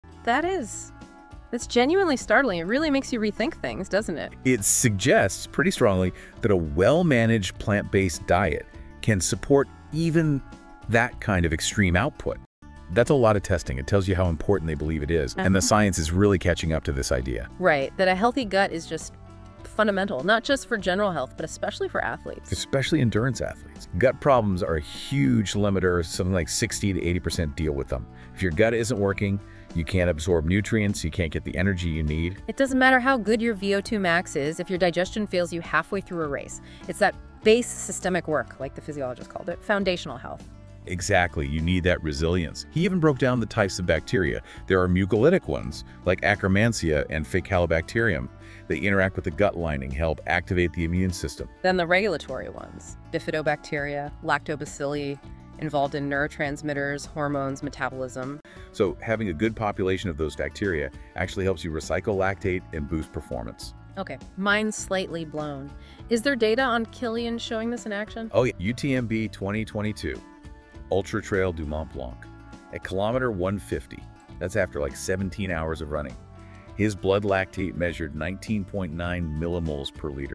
Plant-Based Diet Powers Extreme Hiker? Gut Health Secret REVEALED!
Explore how plant-based diets fuel extreme endurance, the importance of a healthy gut for athletes, and groundbreaking data on lactate recycling. Gut problems can limit performance and understanding your microbiota is crucial. Discover the science behind athletic resilience!